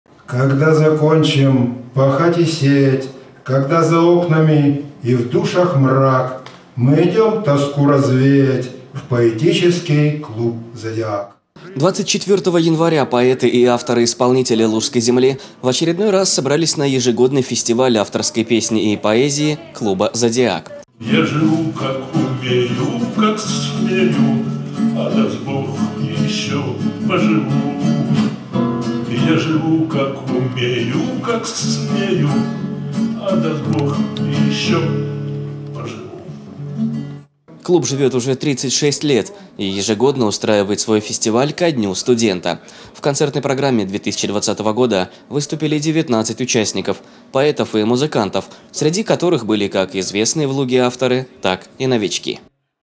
Бардовский фестиваль
24 января поэты и авторы-исполнители лужской земли в очередной раз собрались на ежегодный фестиваль авторской песни поэзии клуба «Зодиак».
Клуб живёт уже 36 лет, и ежегодно устраивает свой фестиваль ко Дню студента. В концертной программе 2020 года выступили 19 участников – поэтов и музыкантов, среди которых были как известные в Луге авторы, так и новички.